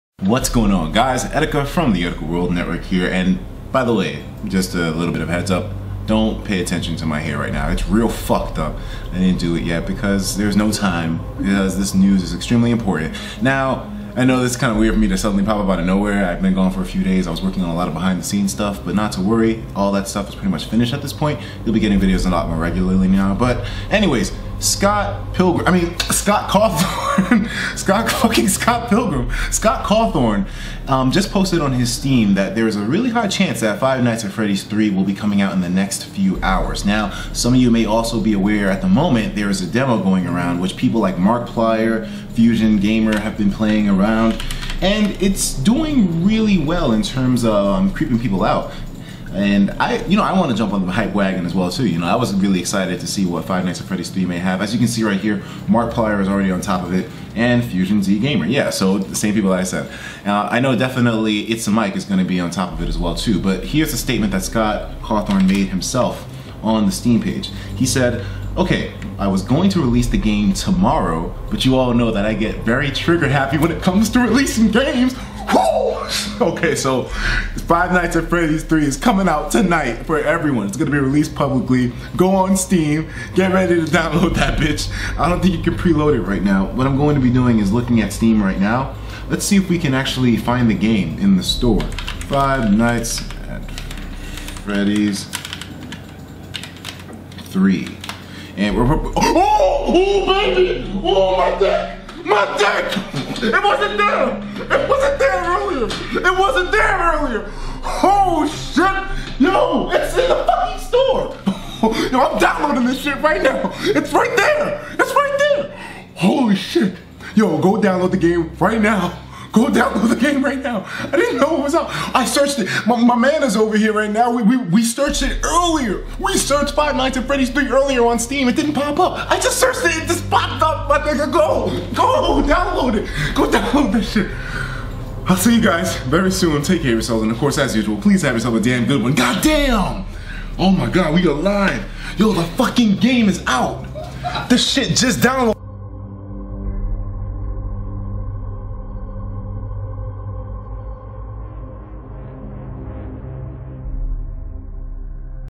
FIVE NIGHTS AT FREDDY'S 3 WAS JUST RELEASED! Surprise Reaction When Checking Steam!!!!! MY DICK!!
Original Upload Date: March 2nd, 2015